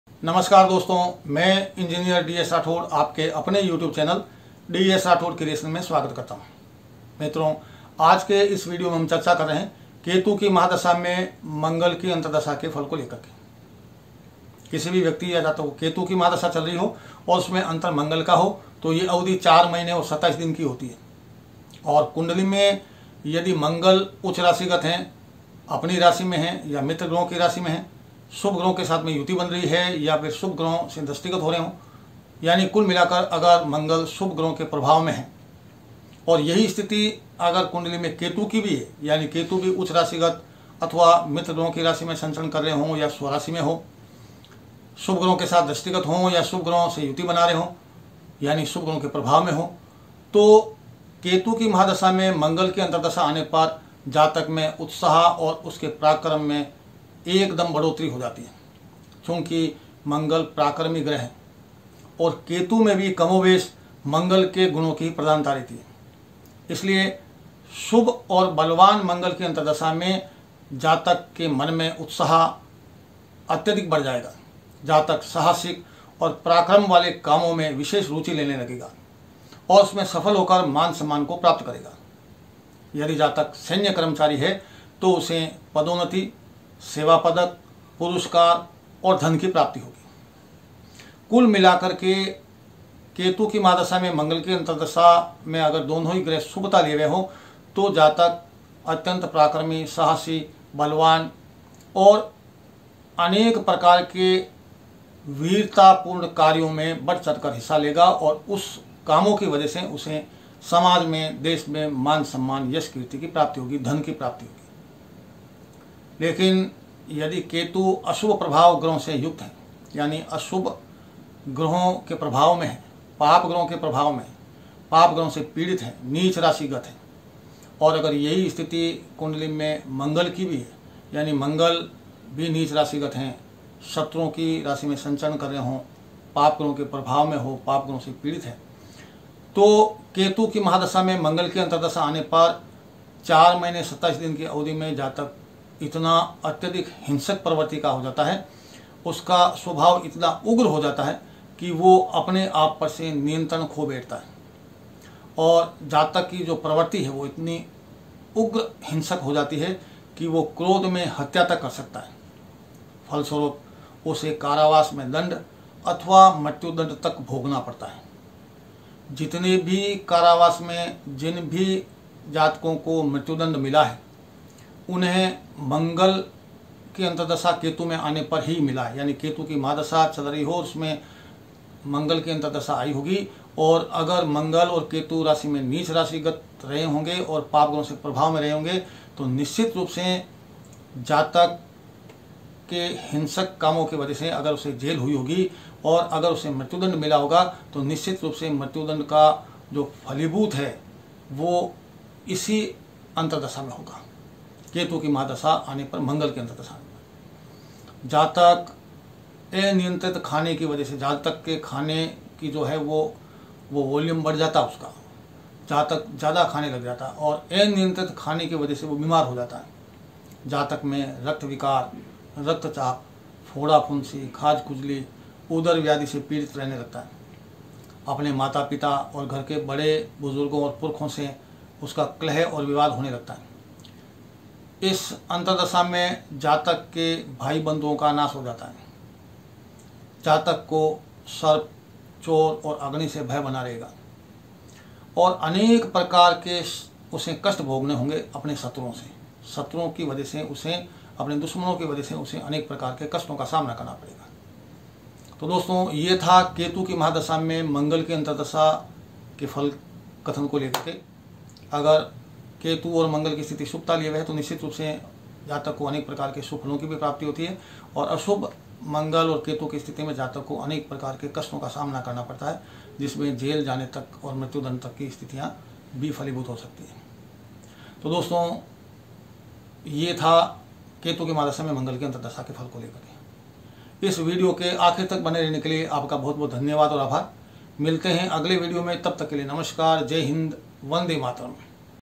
केतु की महादशा में मंगल की अंतरदशा | Ketu Ki Mahadasha Me Mangal Ki Antardasha
केतु की महादशा में मंगल की अंतरदशा का फल़
केतु की महादशा में मंगल की अंतरदशा 4 महीने 27 दिन की होती है। यदि मंगल उच्च, मित्र व स्वराशि में हो, शुभ ग्रहों द्वारा युत व दृष्ट हो एवं केतु भी शुभ ग्रहों के प्रभाव में हो एवं उच्च, मित्र या स्वराशि में स्थित हो तो केतु की महादशा में मंगल की अंतरदशा आने पर जातक में उत्साह और पराक्रम की प्रवृति बढ़ जाती है। मंगल पराक्रमी ग्रह है तथा केतु में भी मंगल के गुणो की प्रधानता है, इसलिए शुभ और बलवान मंगल की अंतरदशा में जातक के उत्साह में वृद्दि हो जाती है। जातक साहसिक और पराक्रम युक्त कार्यों में विशेष रूचि लेता है तथा इनमें सफल होकर मान-सम्मान प्राप्त करता है। यदि जातक सैन्य कर्मचारी है तो पदोन्नति, सेवापदक, पुरस्कार तथा धन मिलता है।
       यदि केतु अशुभ प्रभायुक्त हो एवं मंगल नीच राशि या शत्रु राशि में हो एवं पाप ग्रहों से युक्त या दृष्ट हो तो केतु की महादशा व मंगल की अंतर्दशा आने पर जातक हिंसक प्रवृत्ति का एवं अति उग्र स्वभाव का होता है। जातक अनियंत्रित खाने के कारण पीड़ित हो जाता है। जातक में हिंसक प्रवृति बढ़ जाती है। जातक की हिंसक प्रवृत्ति और ज्यादा उग्र हो जाती है। जातक क्रोध में हत्या तक कर सकता है और फलस्वरुप कारावास दण्ड अथवा मृत्युदण्ड पाता है। जातक अनियंत्रित खाने के कारण बीमार हो जाता है। जातक रक्तविकार, रक्तचाप, फोड़ा-फुन्सी एवं उदर व्याधि से पीडित होता है। अपने माता पिता और घर के बड़े लोगों और पुरखओं से कलह-विवाद होता है। भाई बन्धुओं का नाश होता है। जातक को सर्प, चोर और अग्नि से भय होता है। शत्रु से कष्ट प्राप्त होता है।
DS Rathore